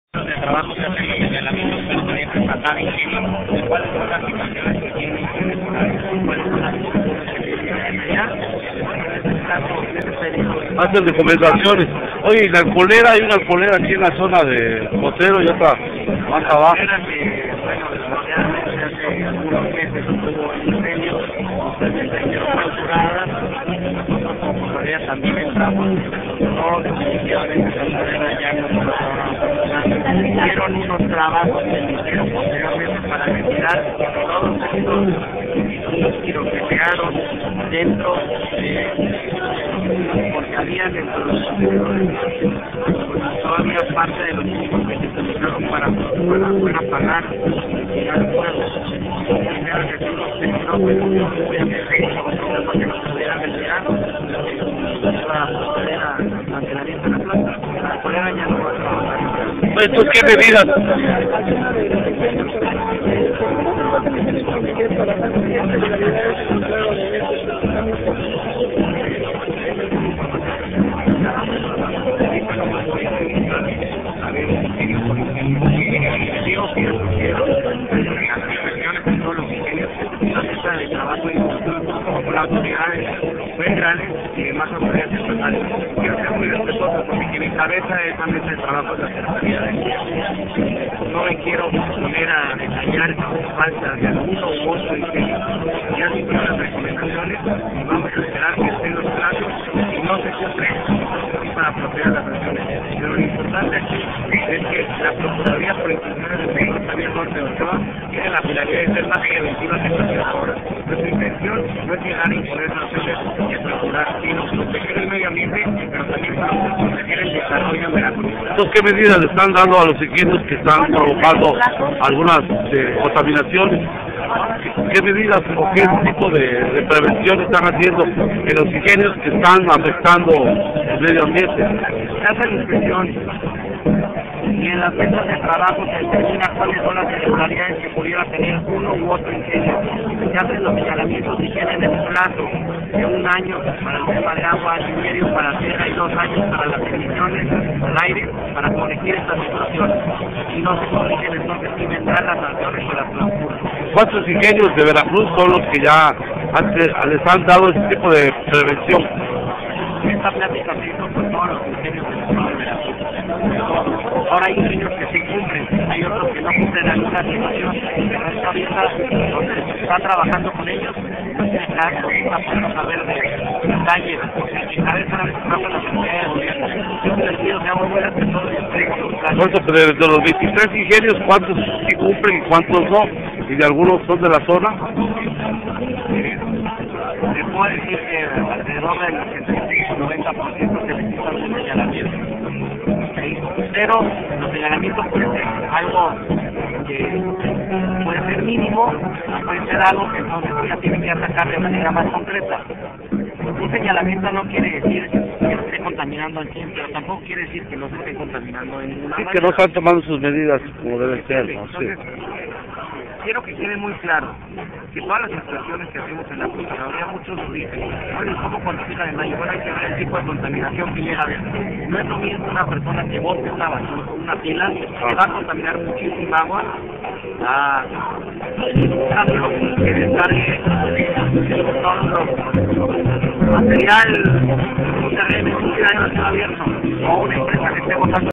Eduardo Aubry de Castro Palomino
UN AÑO COMO PLAZO TIENEN LAS FABRICAS AZUCARERAS PARA CORREGIR ERRORES, CASO CONTRARIO, PUEDEN SER SANCIONADAS, informo la dirección estatal de Protección al Medio Ambiente, que preside el procurador Eduardo Aubry de Castro Palomino, en entrevista con la prensa, luego de cortar el listón inaugural en compañía del alcalde Francisco Portilla Bonilla, de la familia Bravo y demás invitados especiales, de una sucursal más de las gasolineras del "Grupo Gilga" del consorcio --Bravo- de Veracruz, ubicada está en la Luz Francisco I Madero, donde se llevo a cabo una inversión millonaria, así como una fuerte generación de empleos, mismos que vienen a dar certidumbre de empleos y servicios de combustóleo al sector automovilístico y de transporte público y privado aquí en Córdoba, Ver., de la zona centro del Estado.